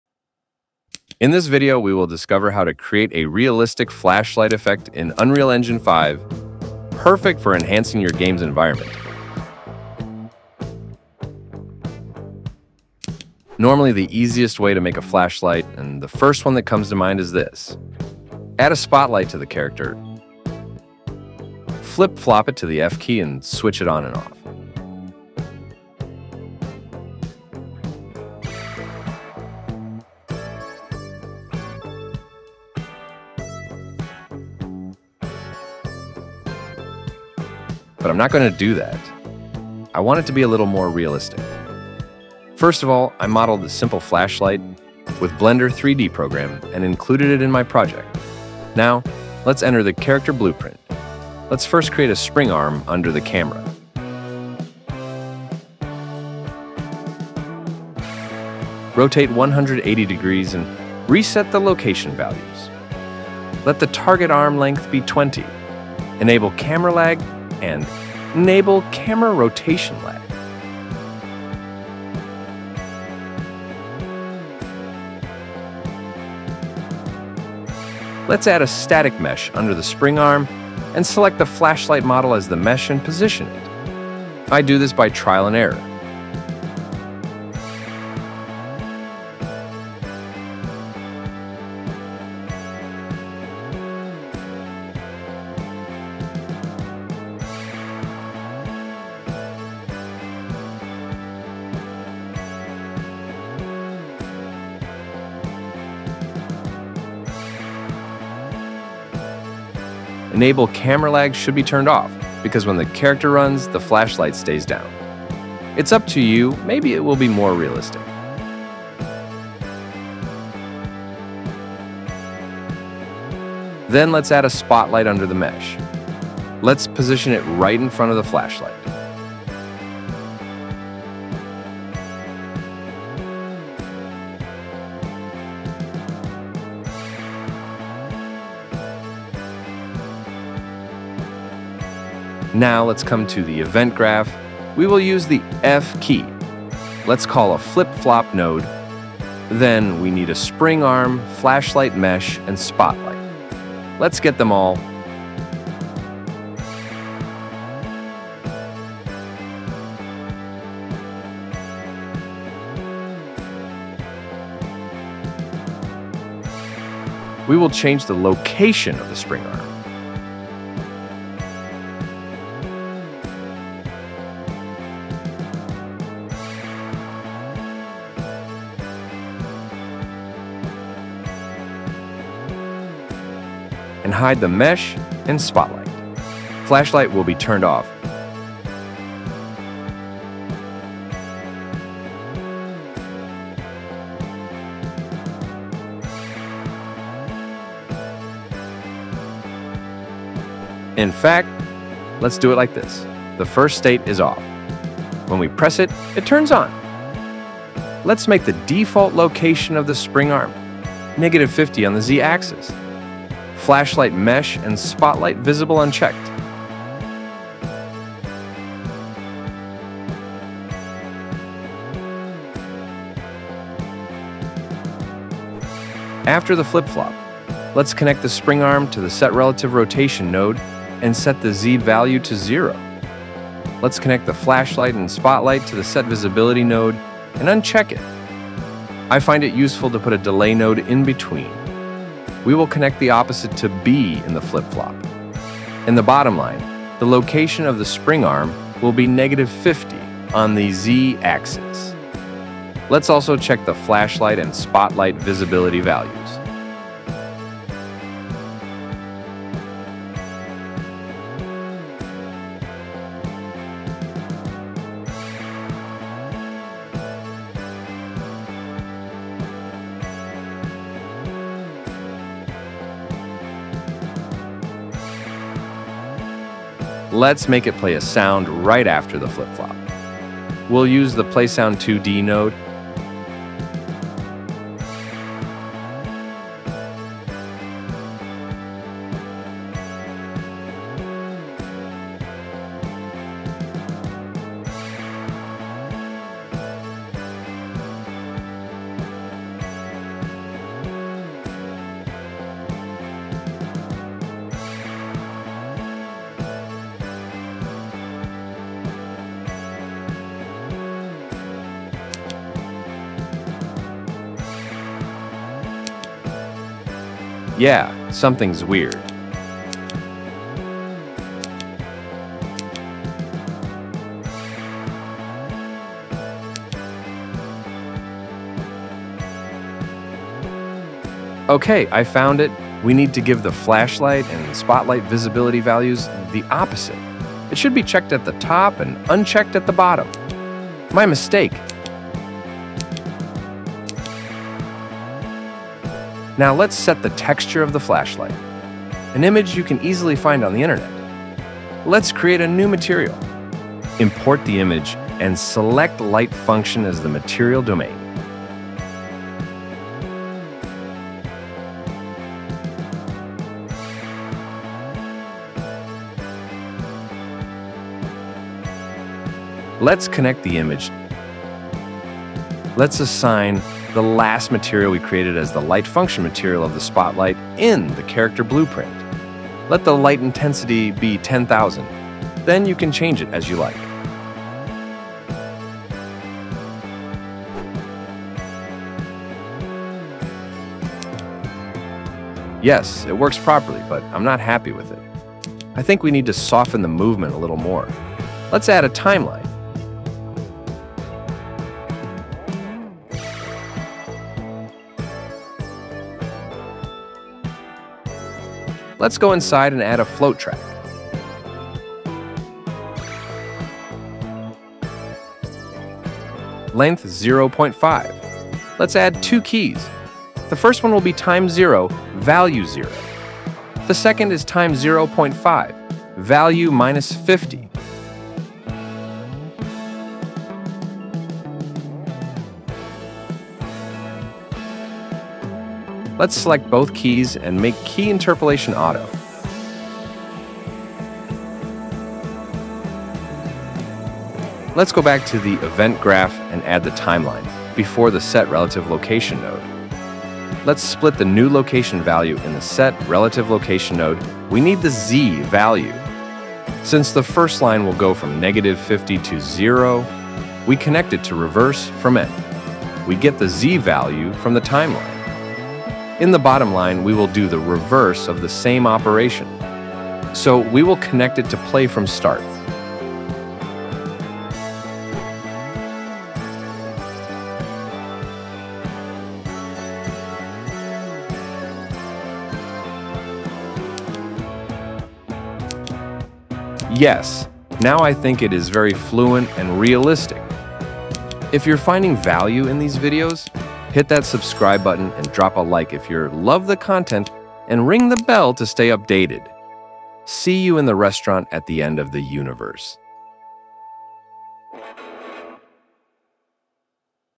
Unreal Engine 5 REALISTIC Flashlight Tutorial - Step by Step Guide for Beginners
Welcome to our comprehensive Unreal Engine 5 Flashlight Tutorial! In this step-by-step guide designed for beginners, we will walk you through the entire process of creating a fully functional flashlight in Unreal Engine 5. Whether you’re working on a game or a simulation, this tutorial will provide you with the essential skills to enhance your projects with dynamic lighting effects.

In this video, you will learn:

How to set up your Unreal Engine 5 project for flashlight implementation.
The basics of creating a flashlight blueprint.
How to adjust the light settings for realistic effects.
Tips for optimizing performance and ensuring smooth gameplay.
Troubleshooting common issues that beginners face.

Join me as we break down each step, making it easy for you to follow along, regardless of your prior experience with Unreal Engine. By the end of this tutorial, you will have a working flashlight that you can use in your own projects!